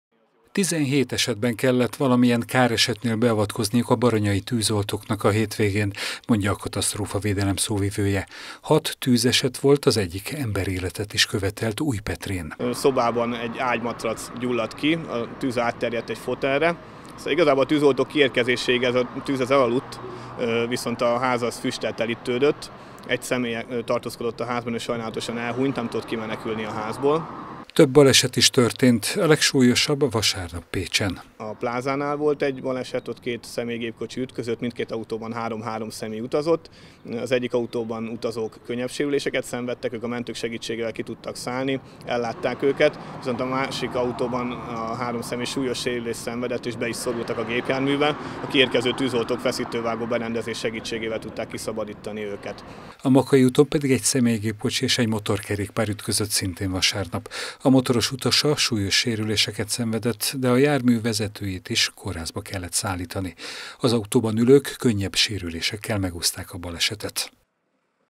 A hétvége mérlege a Katasztrófavédők összesítésében - 2019.04.08.
Halálos tűzeset, több, súlyos sérüléssel járó baleset - ez röviden a hétvége mérlege. Bár kevesebbszer kellett vonulniuk a tűzoltóknak, mint a korábbi hétvégéken, de így is közel 20 esetben kellett beavatkozniuk. Tűzesetből kevesebb akadt az időjárás és a tűzgyújtási tilalom miatt, azonban a füst embert ölt Újpetrén. Pécsen autóroncsból kellett kivágniuk a sérülteket.